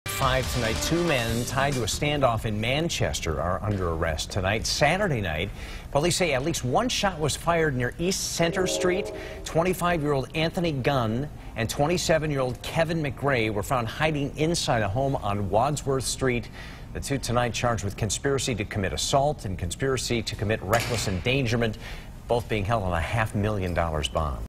PD: 3 people connected to Manchester standoff incident arrested
Two men who officials say were connected to a standoff incident in Manchester were arrested on Tuesday.